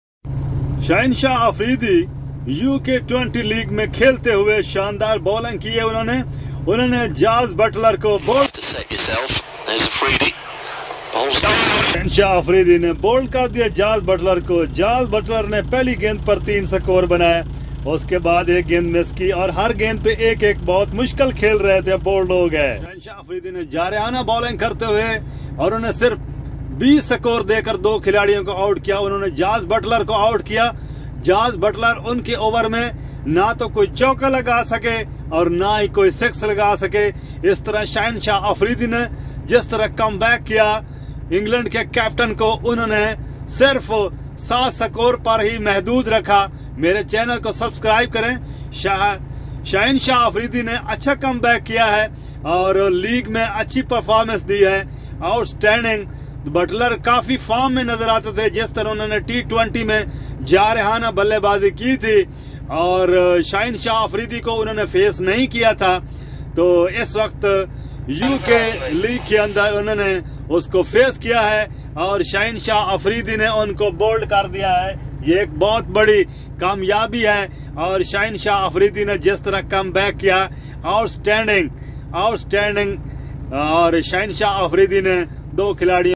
Shaheen Afridi vs Jos Buttler |Notts outlaws vs Lancashire lightning
shaheen shah Afridi bowled to Jos Buttler in the last bowl of his second over .shaheen shah is a very agracive bowler in the world .